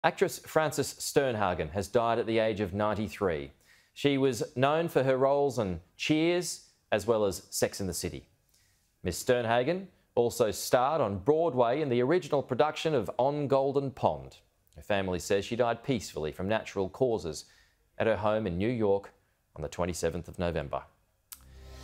Sex and the City actor Frances Sternhagen dies aged 93
Actress Frances Sternhagen has died at the age of 93.

Ms Sternhagen was best known for her roles on 'Cheers' and 'Sex and the City.'

She also starred on Broadway in the original production of 'On Golden Pond.'

Her family said she died peacefully from natural causes in her home in New York on November 27.

Ms Sternhagen’s son John Carlin shared a tribute on his Instagram page, saying his mother “was beloved by many. I’m very lucky I was able to call her my mom, my friend, my song and dance partner.”